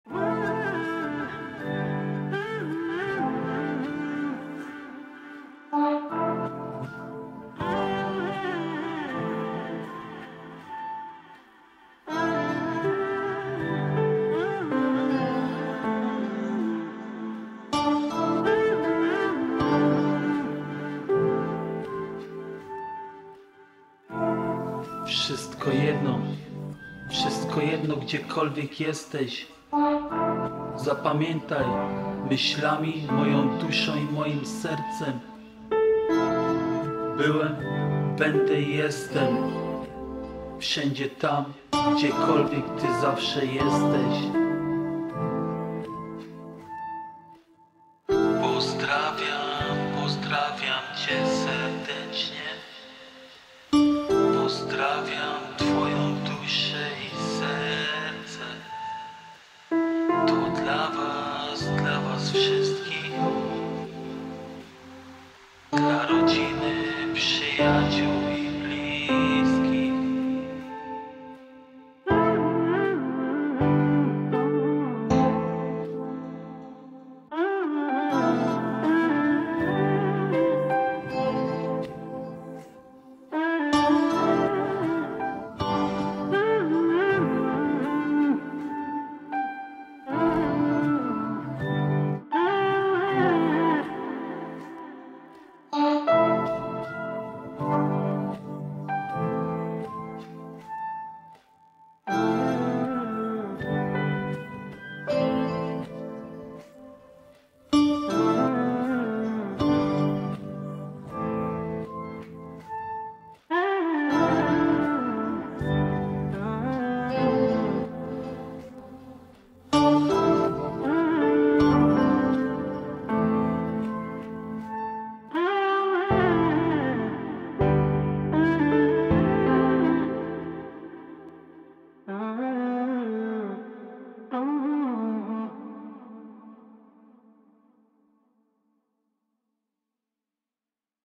GDZIEKOLWIEK JESTES 2024
DLA KAZDEGO KTO KOGOS W ZYCIU STRACIL KTO BYL MU WAZNY KAZDEGO KOMU KOGOŚ BRAK
KAZDEGO KOMU JEST KTOS WAZNY